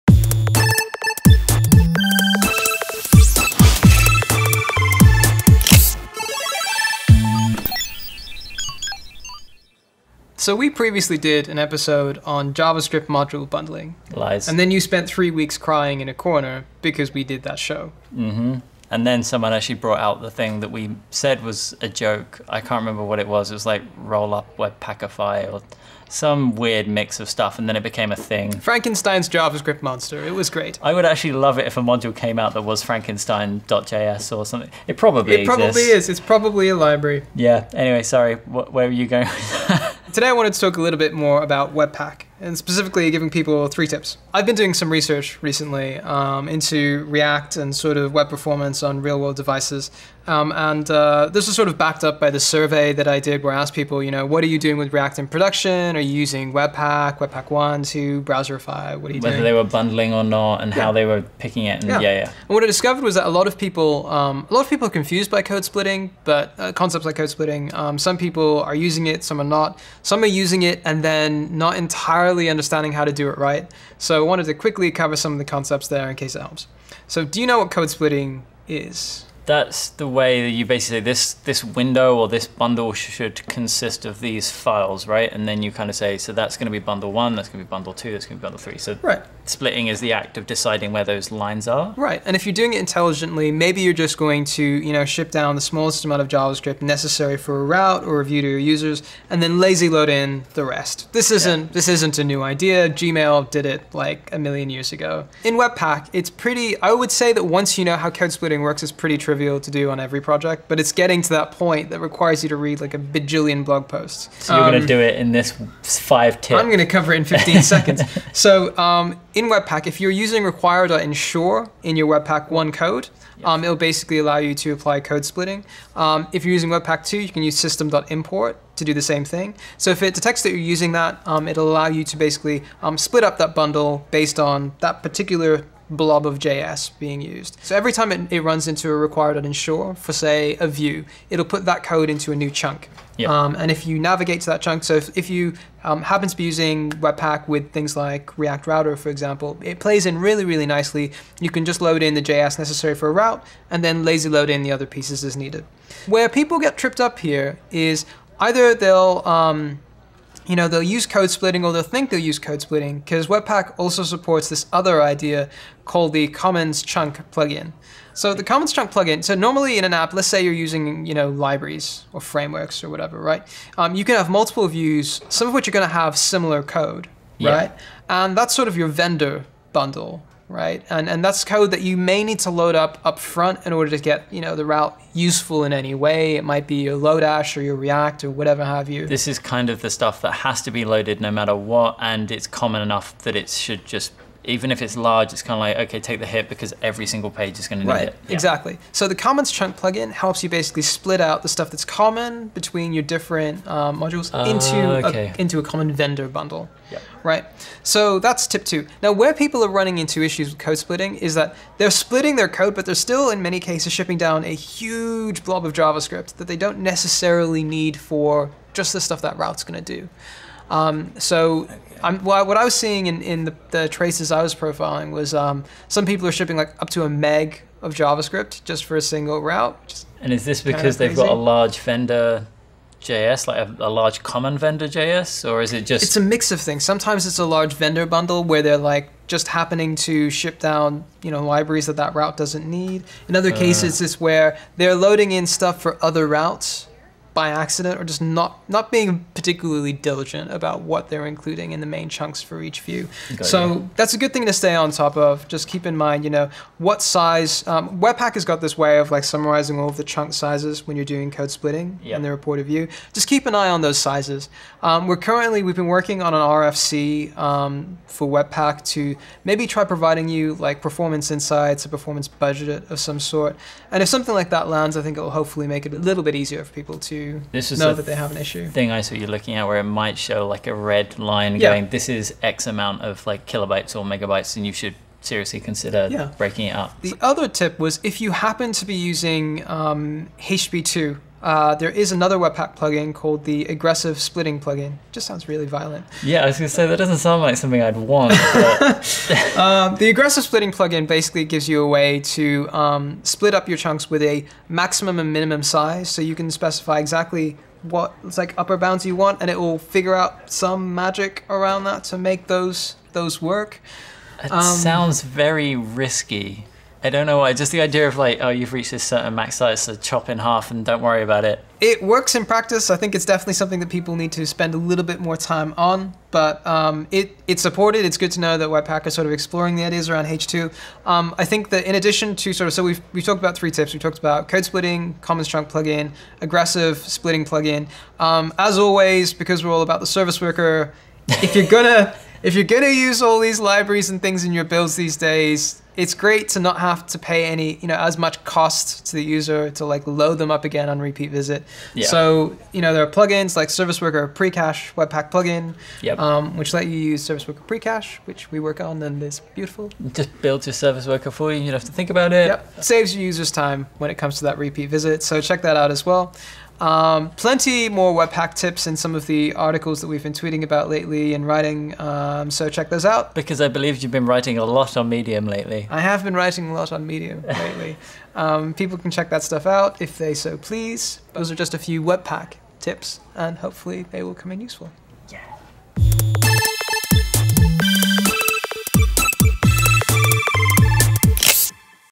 Code-splitting your way to better perf with Webpack in Totally Tooling Tips (S3, E15)
Using Webpack want to learn more about code splitting? Then check out this weeks Totally Tool Tips where Addy gives you his top tips and Matt, unhelpfully, questions it. totes a webpack noob.